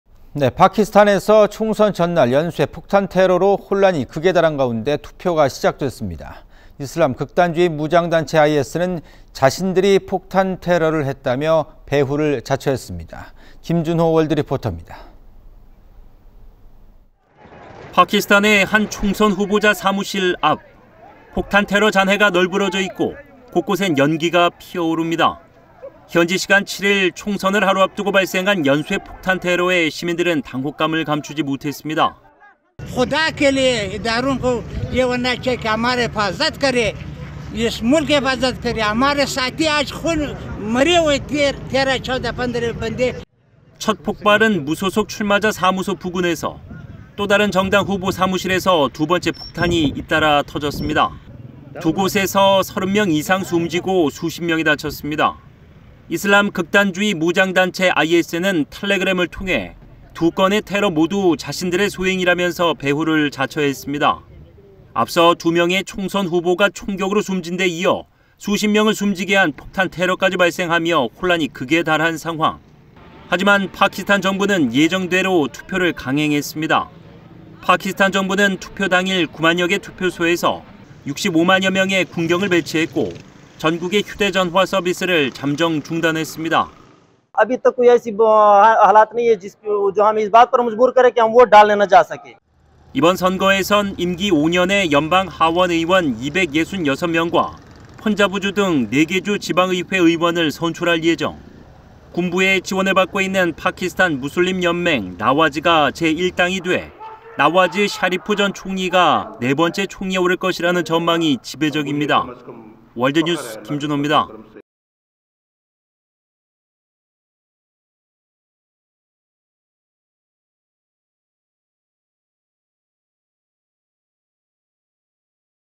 '테러 얼룩' 파키스탄 총선투표 강행
【앵커】
파키스탄에서
총선 전날 연쇄 폭탄 테러로
혼란이 극에 달한 가운데
투표가 시작됐습니다.
이슬람 극단주의 무장단체, IS는
자신들이 폭탄 테러를 했다며
배후를 자처했습니다.
 김준호 월드리포터입니다.

【기자】

파키스탄의 한 총선 후보자 사무실 앞.

폭탄 테러 잔해가 널브러져 있고, 곳곳엔 연기가 피어오릅니다.

현지시간 7일 총선을 하루 앞두고 발생한 연쇄 폭탄 테러에 시민들은 당혹감을 감추지 못했습니다.

[페로즈 칸 / 발루치스탄 주민 : 이렇게 모든 것이 파괴돼 있었어요. 파키스탄 당국에서 시민과 국가를 보호해야 합니다.]

첫 폭발은 무소속 출마자 사무소 부근에서, 또 다른 정당 후보 사무실에서 두 번째 폭탄이 잇따라 터졌습니다.

두 곳에서 30명 이상 숨지고 수십 명이 다쳤습니다.

이슬람 극단주의 무장단체 IS는 텔레그램을 통해 두 건의 테러 모두 자신들의 소행이라면서 배후를 자처했습니다.

앞서 2명의 총선 후보가 총격으로 숨진데 이어, 수십 명을 숨지게 한 폭탄테러까지 발생하며 혼란이 극에 달한 상황.

하지만 파키스탄 정부는 예정대로 투표를 강행했습니다.

파키스탄 정부는 투표 당일 9만여 개 투표소에 65만 명의 군경을 배치했고, 전국의 휴대전화 서비스를 잠정 중단했습니다.

[마울라나 라힘 울라 / 유권자 : 군대는 법질서 유지를 위해 배치된 것입니다. 우리가 투표하러 가지 않을 이유가 없습니다.]

이번 선거에선 임기 5년의 연방 하원의원 266명과 펀자브주 등 4개주 지방의회 의원을 선출할 예정.

군부의 지원을 받고 있는 파키스탄무슬림연맹-나와즈가 제1당이 돼 나와즈 샤리프 전 총리가 네 번째 총리에 오를 것이라는 전망이 지배적입니다.

월드뉴스 김준호입니다.